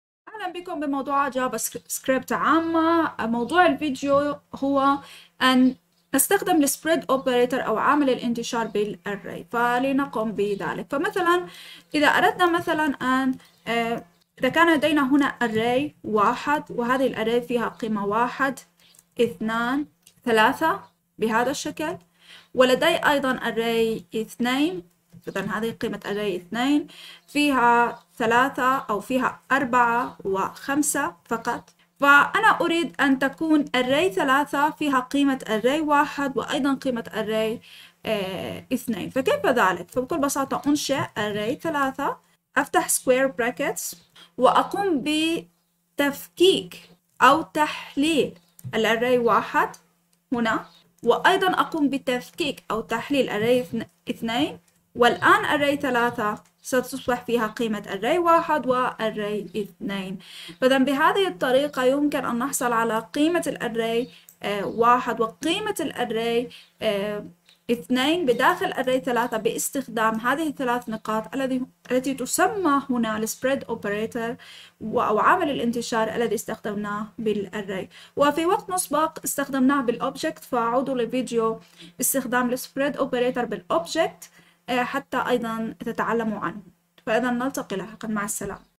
عامل الانتشار في المصفوفات | Spread Operator in Arrays
عند العمل مع المصفوفات في لغة JavaScript، قد تحتاج في كثير من الأحيان إلى دمج عدة مصفوفات في مصفوفة واحدة. توفر لك عامل الانتشار (المعروف بالعلامة ...) طريقة موجزة وفعالة لتحقيق ذلك.
فهرس الدرس:
00:00:00  مقدمة الدرس
00:00:11 Spread Operator